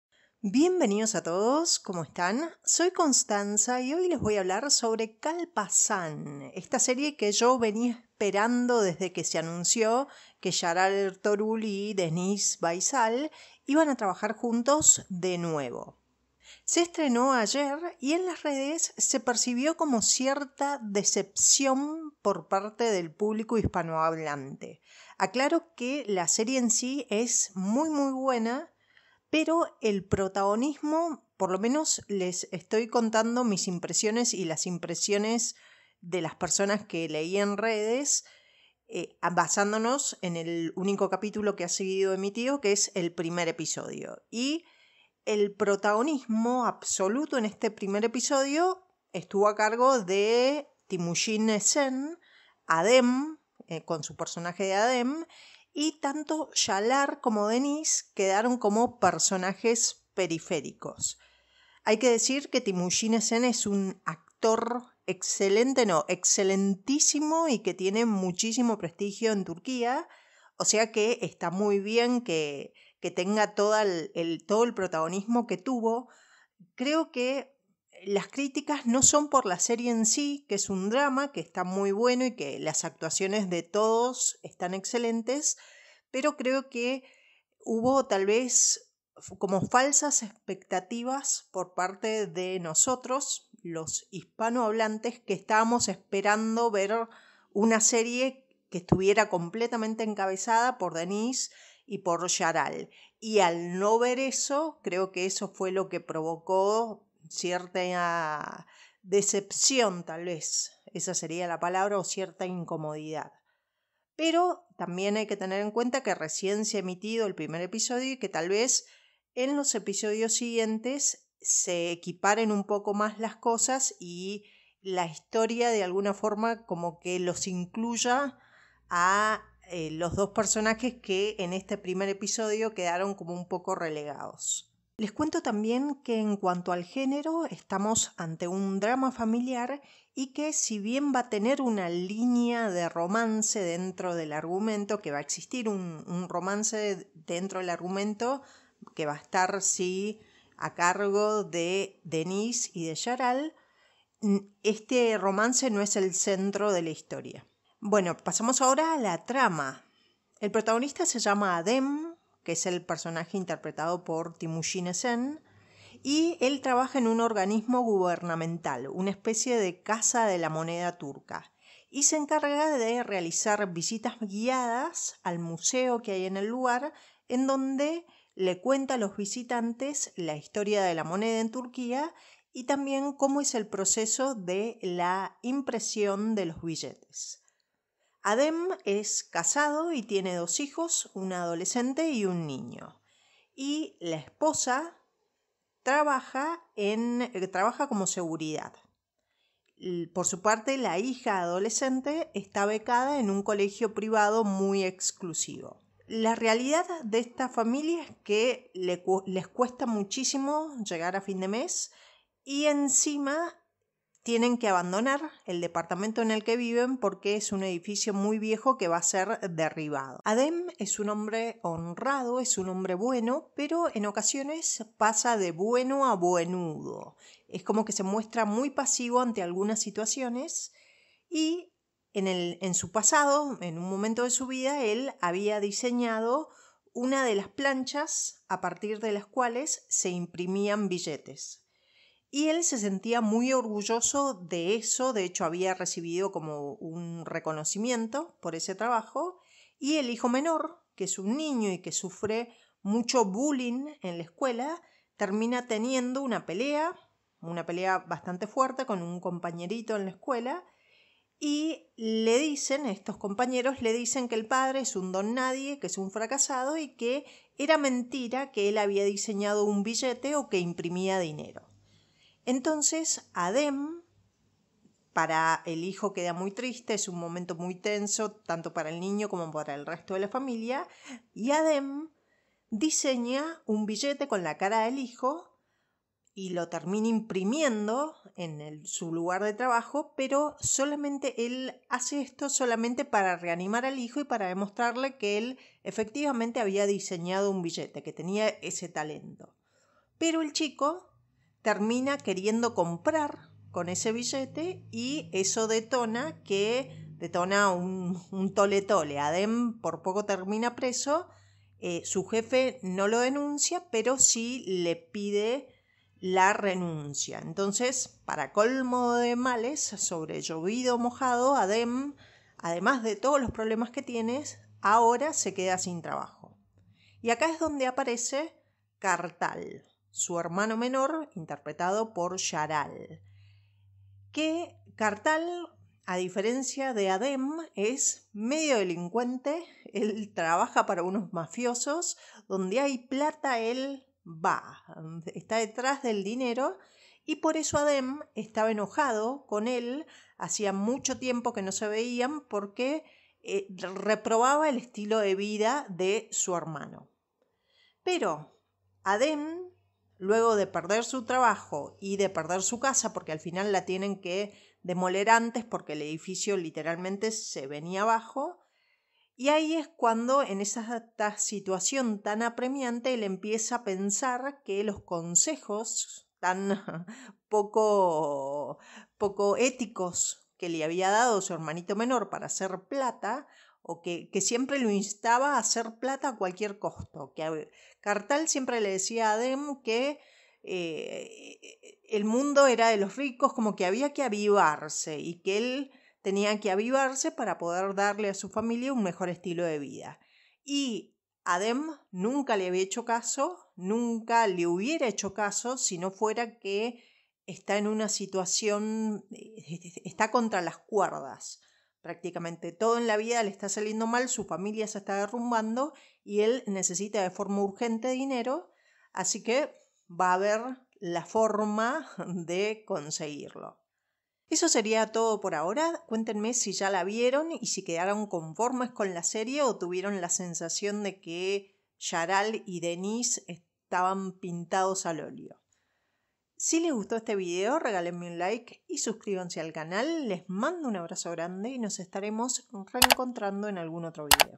💥Nueva serie TURCA KALPAZAN Capítulo 1 español RESEÑA 🔥
🧧 Kalpazan nueva serie turca con Caglar Ertugrul y Deniz Baysal.

¿Te gustaría leer mis novelas?
📙 Cuando sea flaca- Comedia Romántica
⚜️Canal de reseñas de películas y series románticas (No turcas)